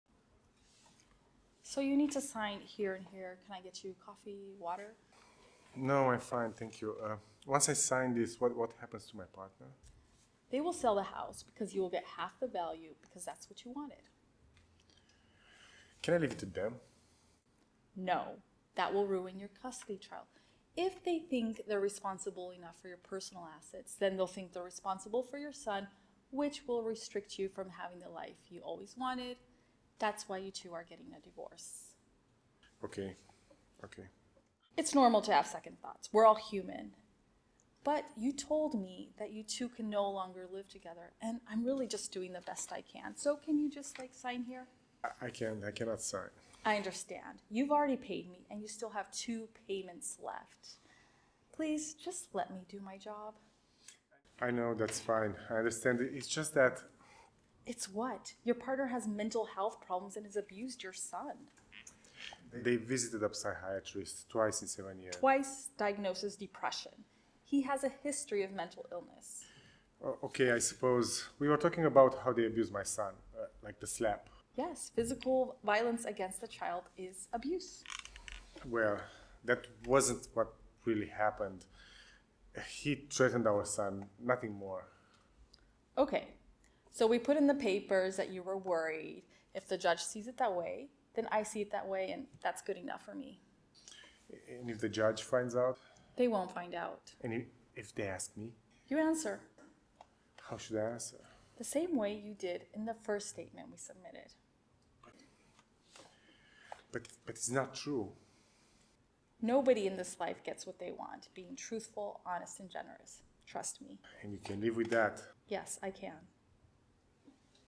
A scene from a movie about divorce
I played the lawyer to get her client to sign the divorce papers.